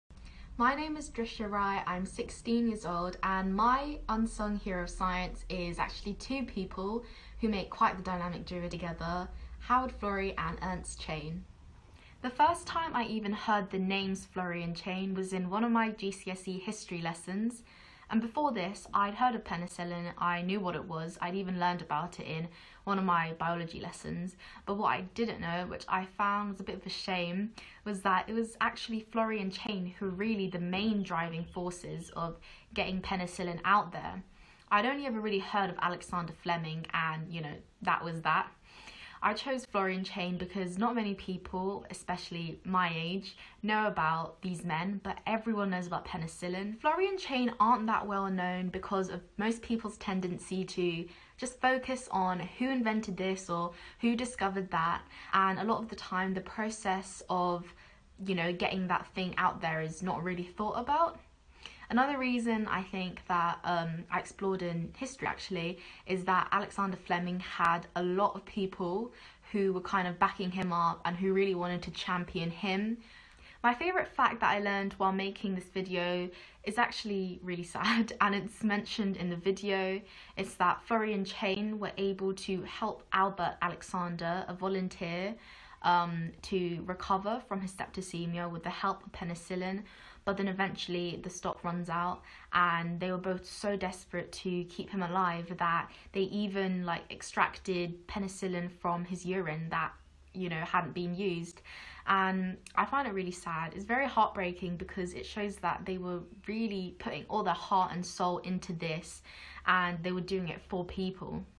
Howard Florey & Ernst Chain: Meet the creator
Drishya created this year's winning video in the Unsung Heroes of Science competition. Here she explains why she chose Florey & Chain and what we can learn from them.